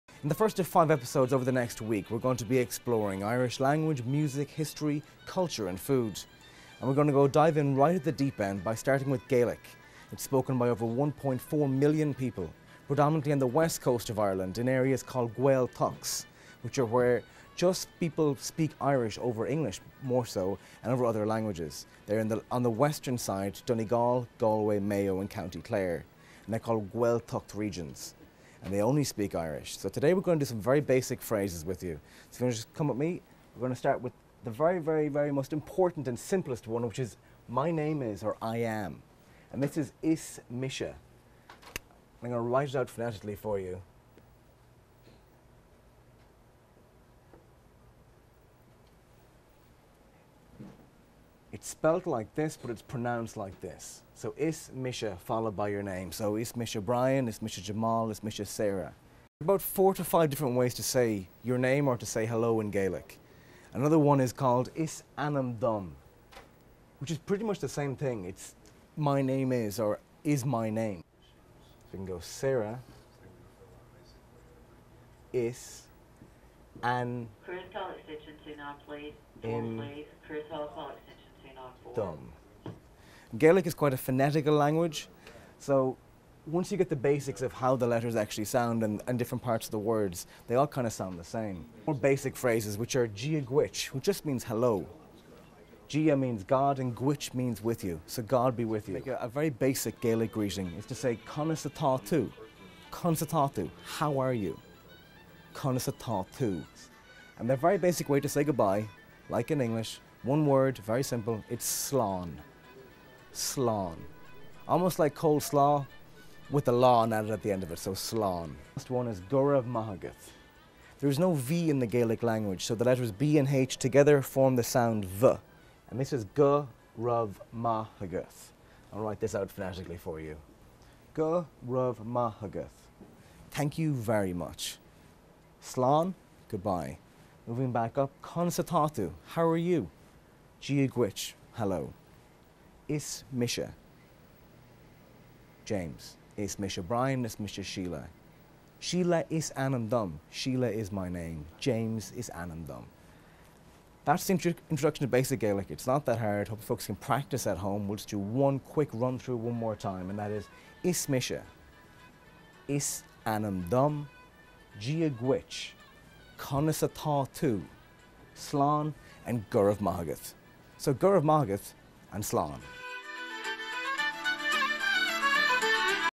Intro to Gaelic
Are you ready for St. Patty's Day? Our Irish reporter James Mahon can help you brush up on your Gaelic!